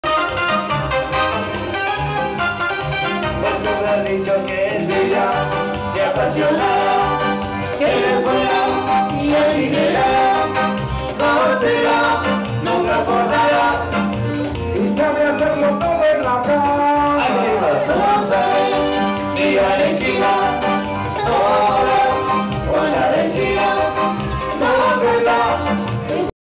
BAILE
mi pata chavez.. bailando con mi tia rosa.. en el cumple d mama